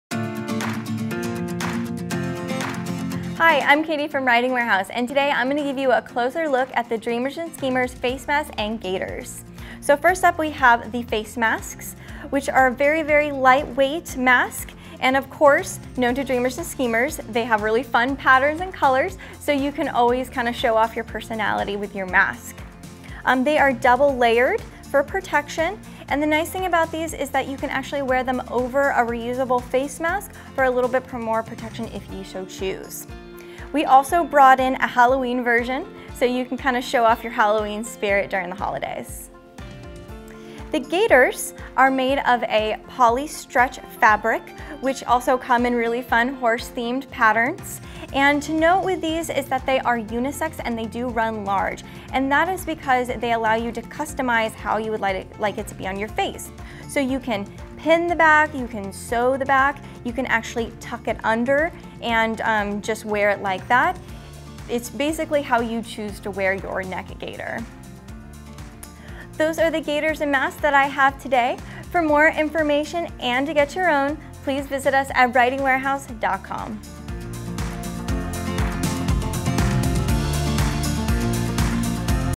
RW Insider's Vlog: New Dreamers & Schemers Masks & Gaiters
Keep yourself protected with these fun Dreamers & Schemers Equestrian Masks and Gaiters! Made from 100% cotton and designed with cute horse patterns, these masks are a must-have during COVID times.